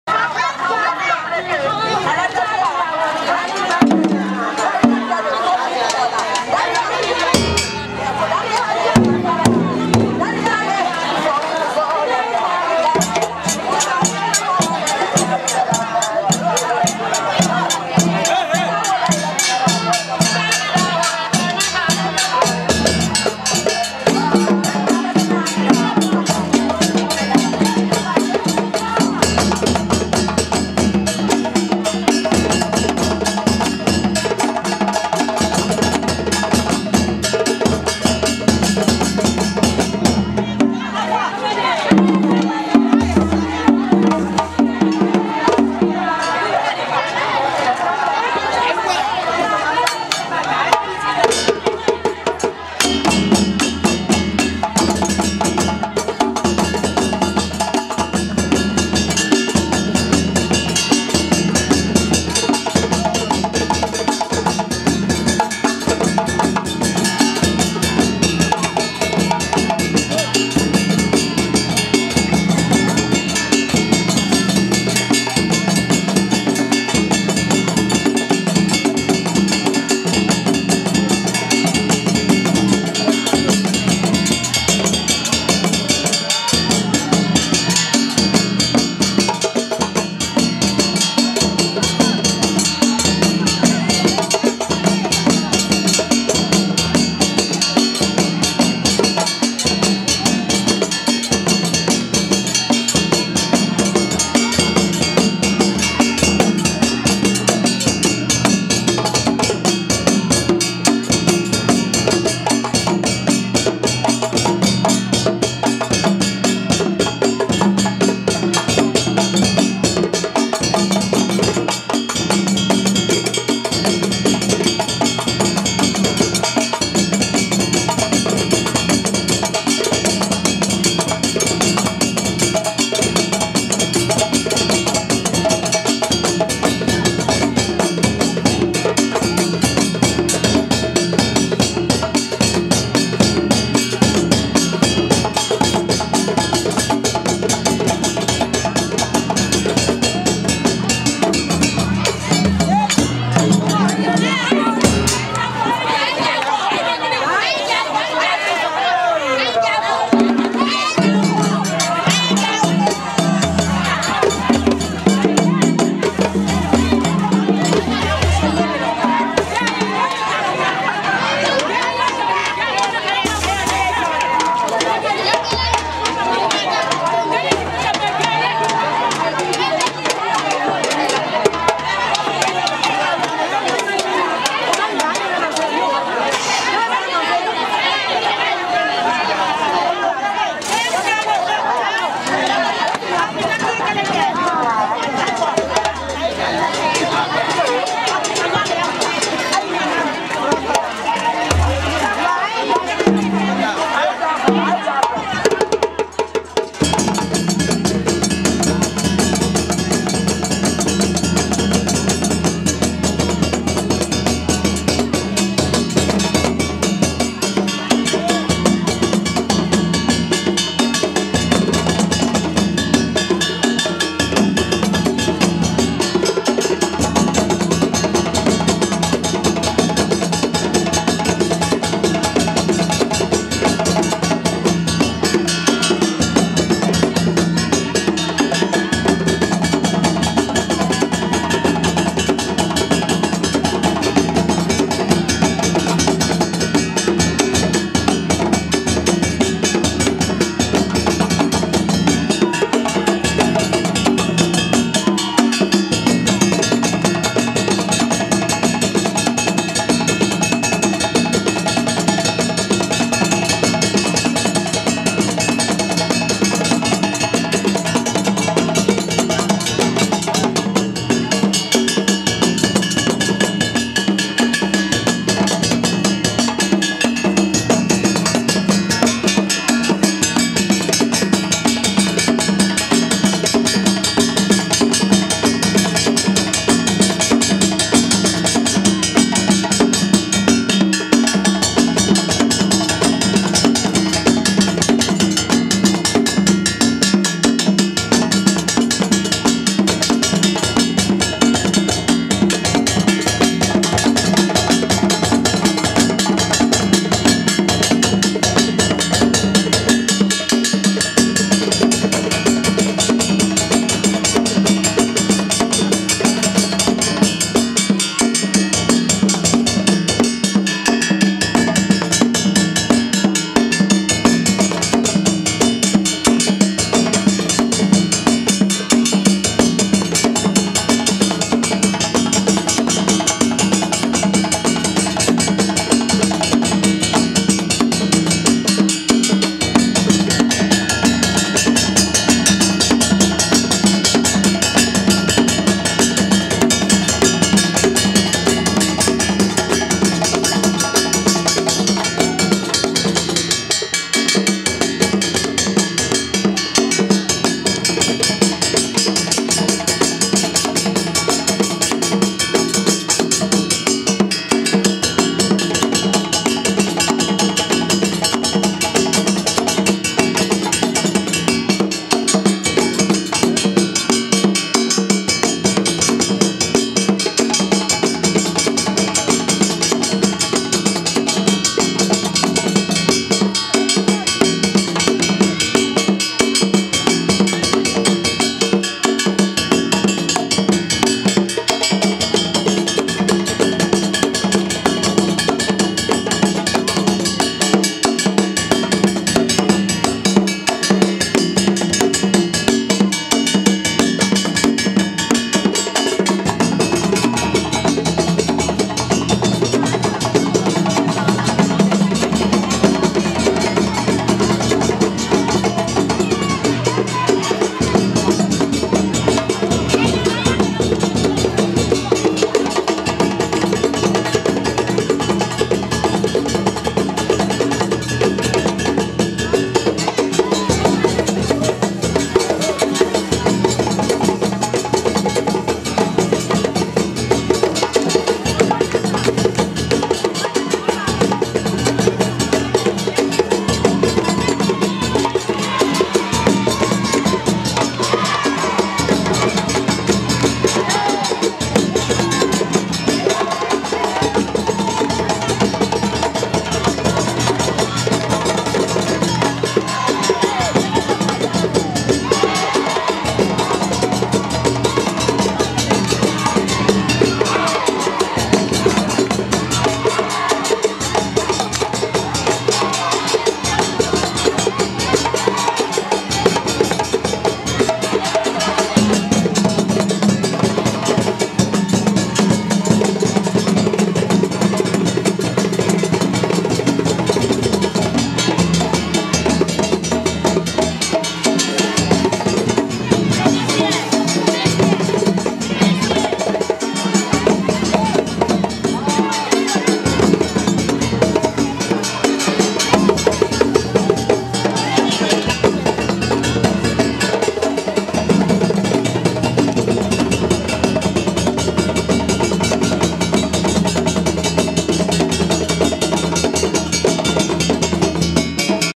Boda en Kouroussa.avi
Hola a todos!! este es un vídeo de una boda para promocionar a los músicos con los que hemos estado aprendiendo en kouroussa(guinea), durante dos meses.
Djembe-1MORIDENI,Djembe-2,OSUMANE,Sangban-FRACAS-Kenkeni,NAMORI y Doundoun-ABOUBACAR.
En el caso de que queráis visitarlos, escribirme y os daré el numero de ellos.

saludos!!